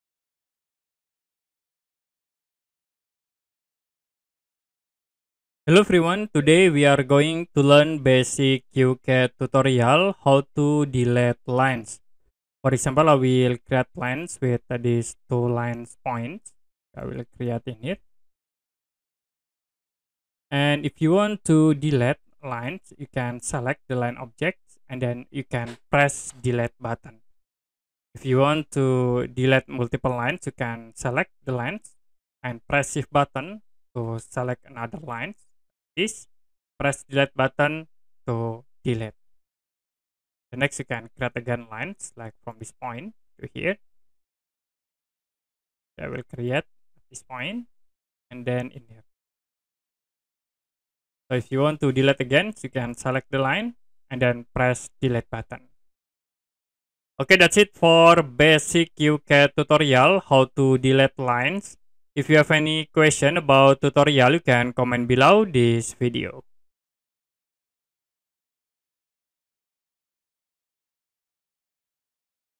How To Delete Lines In QCAD
To delete lines in QCAD, you can select the line or lines you want to delete and use the "Delete" key on your keyboard or the "Delete" option in the Edit menu. This tutorial will show you how to easily delete lines in QCAD.